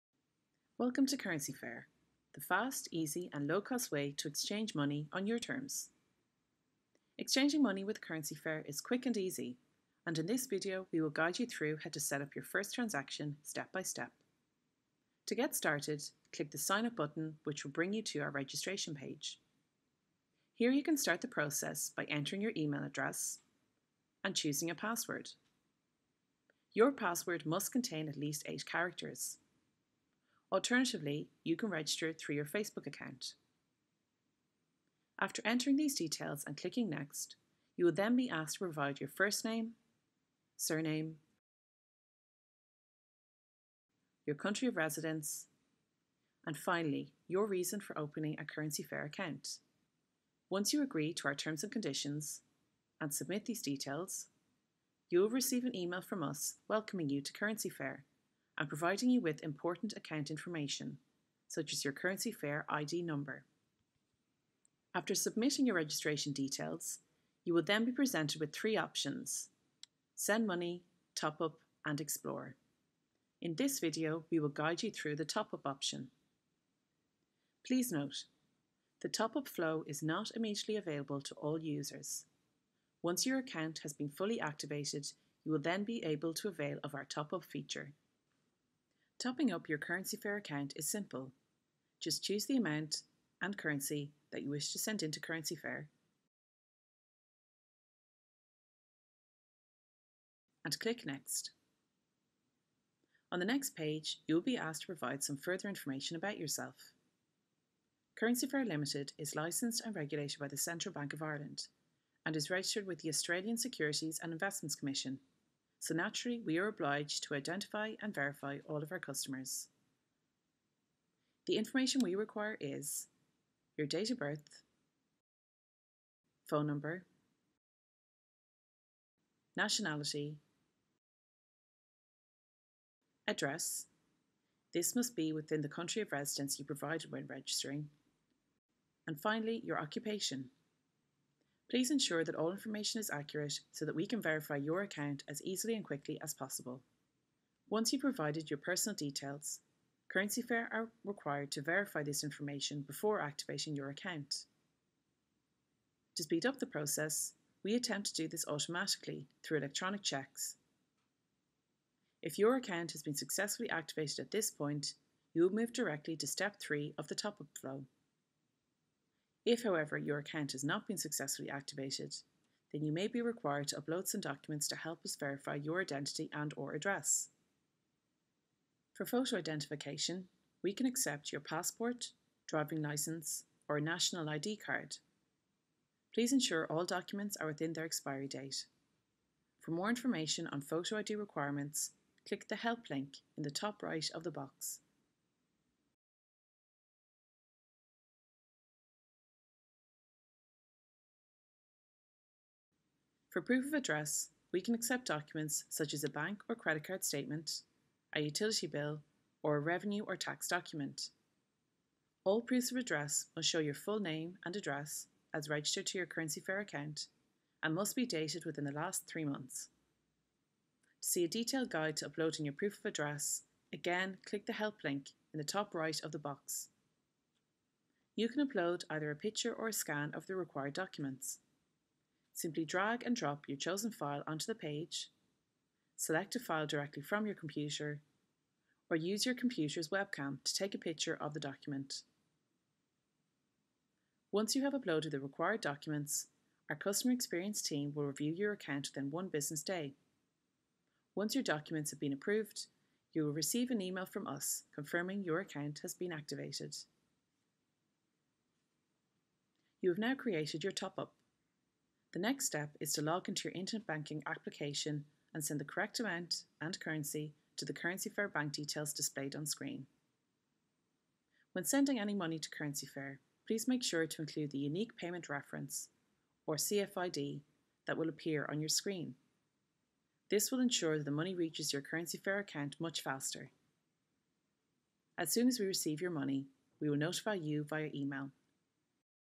How to set up a CurrencyFair Account - Top Up
Setting up a CurrencyFair account is quick and easy. Our video guide will take you through the process step by step.